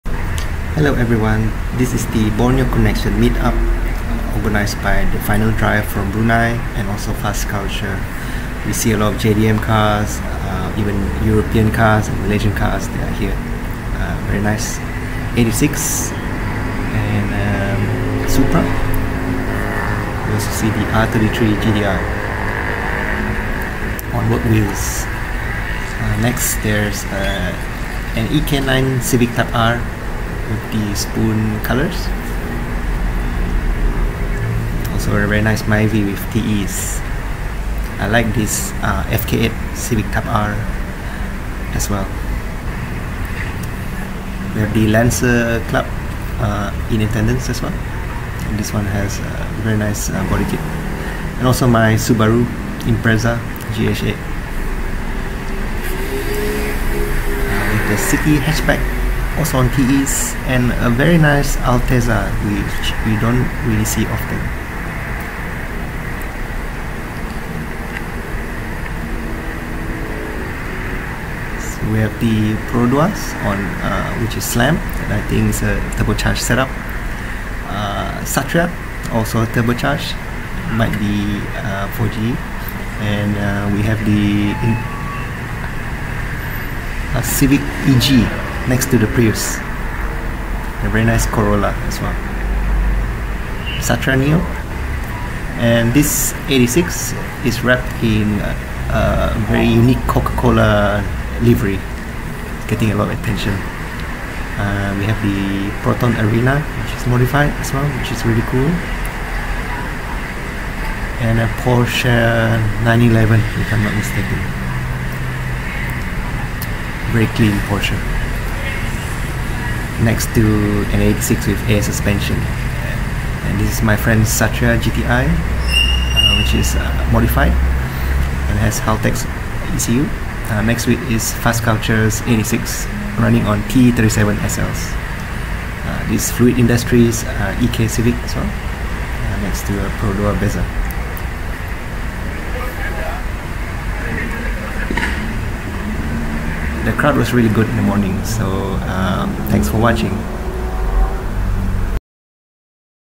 Borneo Connection Meet Up
The Borneo Connection Meet Up was organized by thefinaldrive and Fast Culture at Petros Multi-fueling station, Kuching Sarawak Borneo. There was a great mix of JDM, Euro and Malaysian cars.